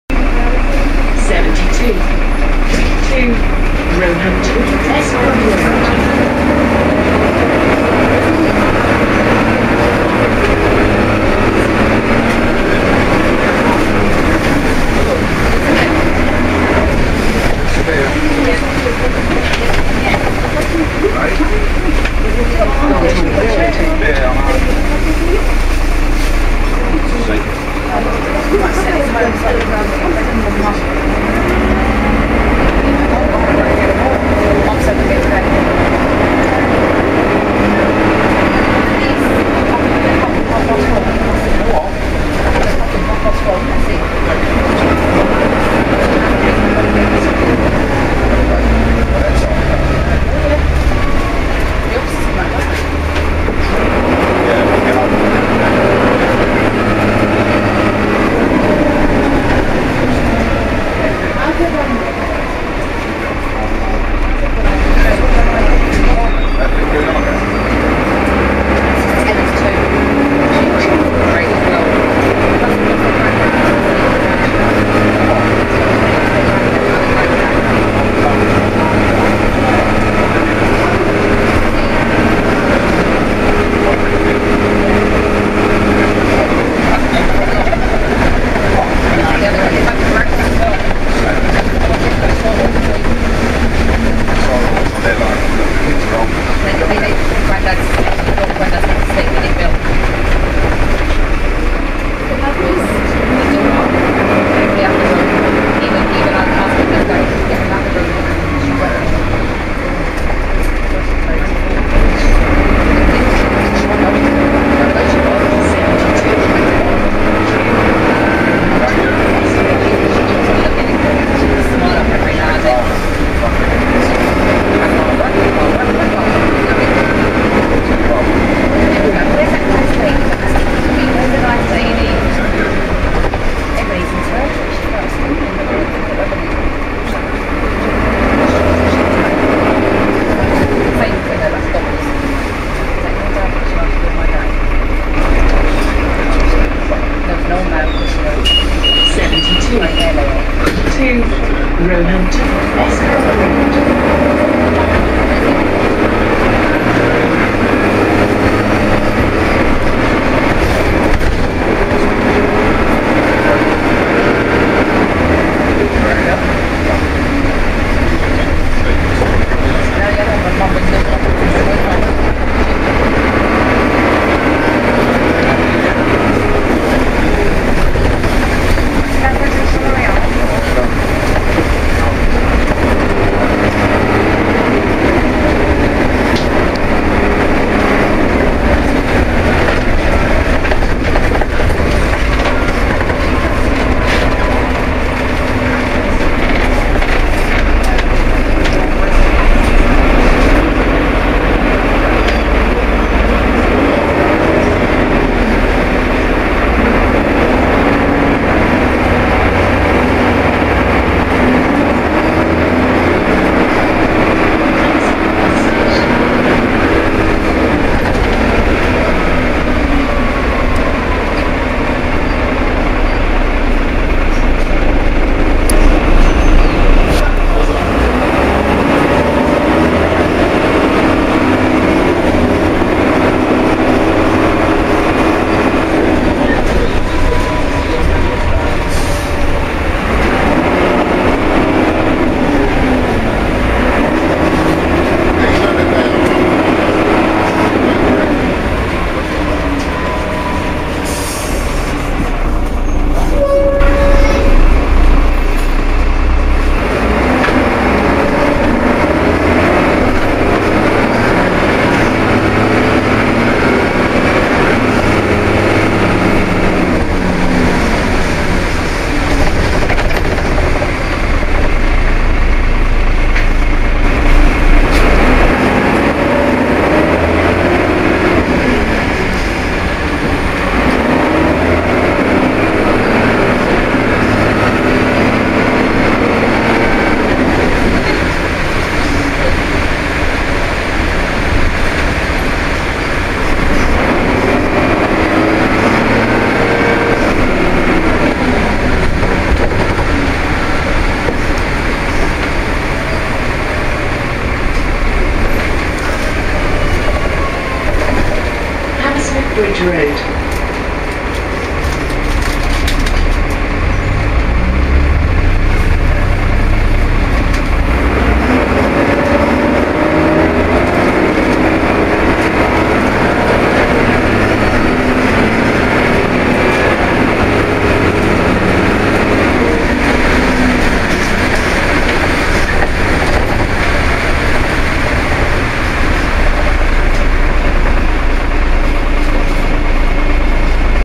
Journey On The 72 (DPS539,Y539XAG) Dennis Dart SLF 10.2m/Plaxton Pointer 2
DPS539 serving Route 72 in Hammersmith.
Route 72: East Acton - Roehampton, Bessborough Road.
Operated by London United - Shepherd's Bush (S) Garage